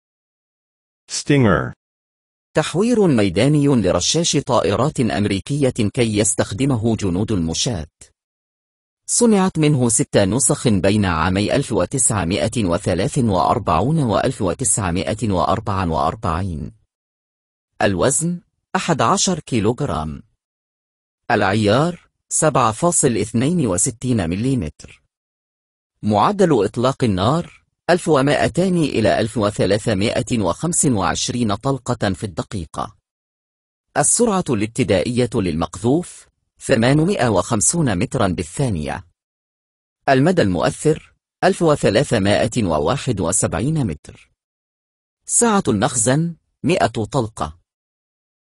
stinger
تحوير ميداني لرشاش طائرات أمريكية كي يستخدمه جنود المشاة
صنعت منه 6 نسخ بين عامي 1943 و 1944
الوزن 11 كيلوغرام
العيار 7.62 مليمتر
معدل إطلاق النار 1200 الى 1325 طلقة في الدقيقة
السرعة الابتدائية للمقذوف 850 متر بالثانية
المدى المؤثر 1371 متر
سعة المخزن 100 طلقة